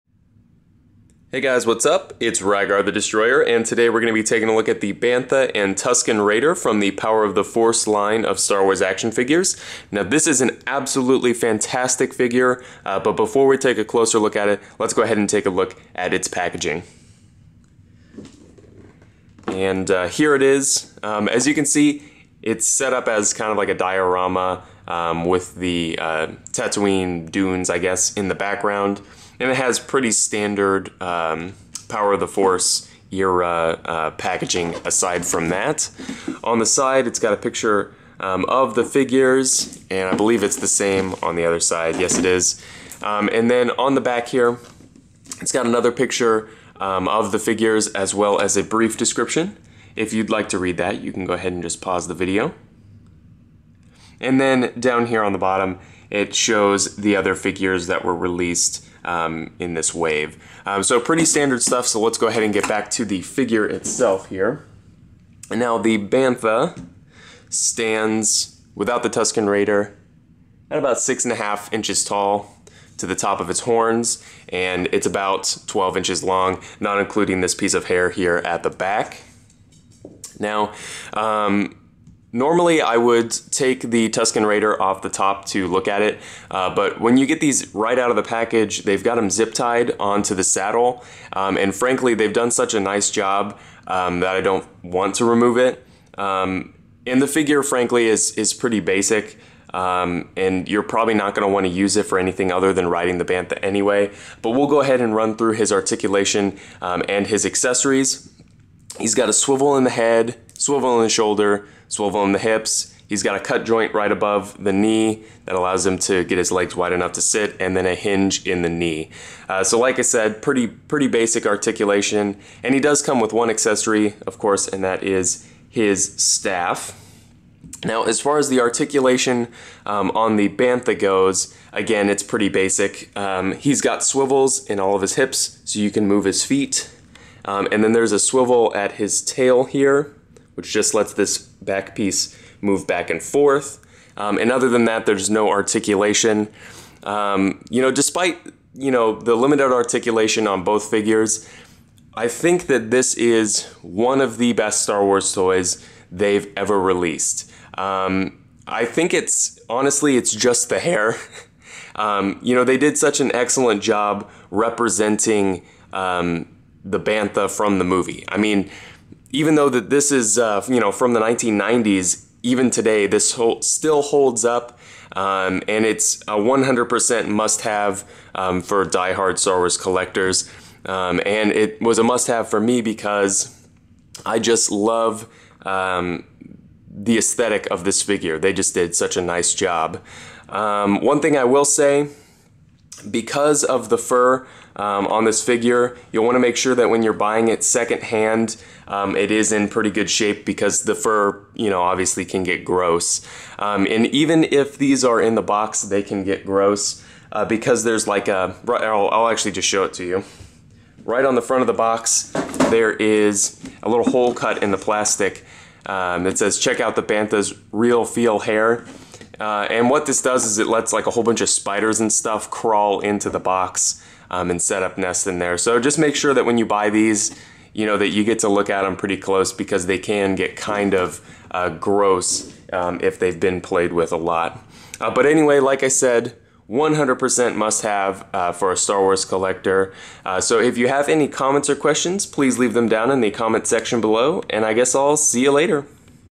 Star Wars Action Figure Review: Bantha and Tusken Raider
A quick look at the Bantha and Tusken Raider from the Power of the Force line.